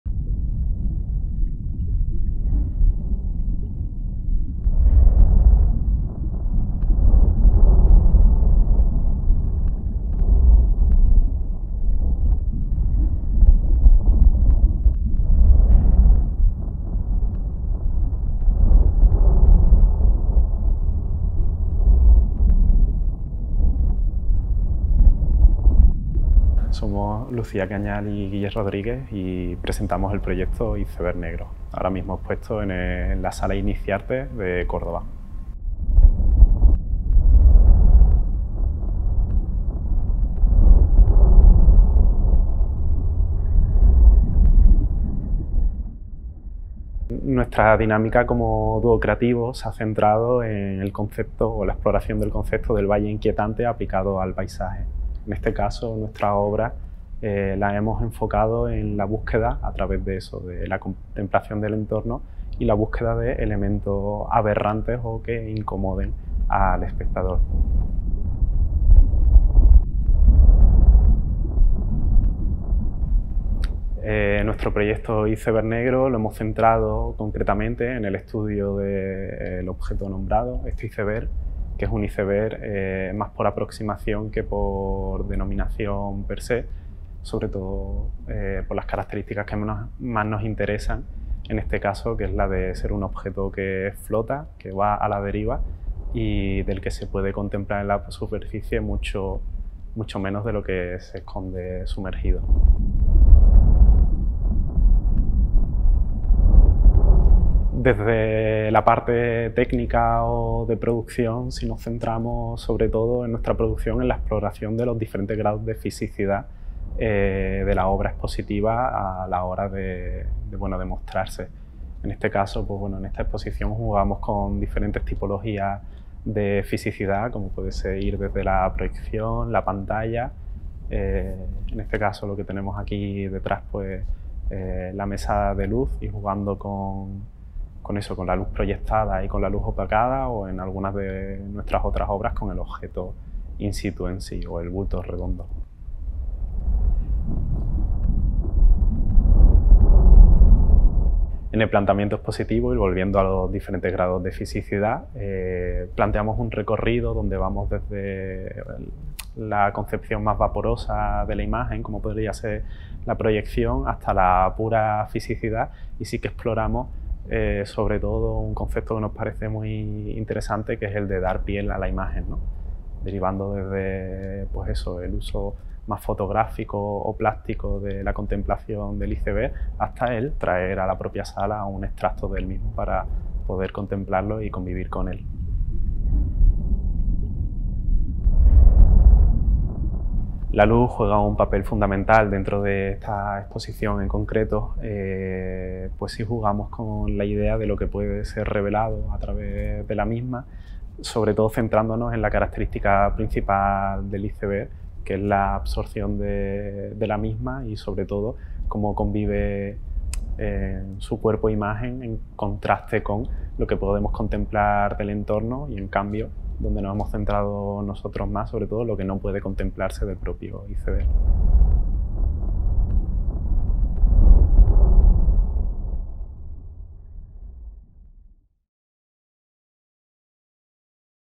Lucía Cañal y Guille Rodríguez 'Iceberg Negro' | INICIARTE
En este vídeo, Lucía Cañal y Guille Rodríguez nos explican la muestra 'Iceberg Negro', que se plantea como búsqueda estética sobre la tensión y relación que genera un cuerpo extraño en el medio natural.

Iceberg negro, de Lucía Cañal y Guille Rodríguez, puede visitarse hasta el 26 de noviembre en el Espacio Iniciarte de Córdoba (Calle Capitulares 2 y calle Pedro López 5).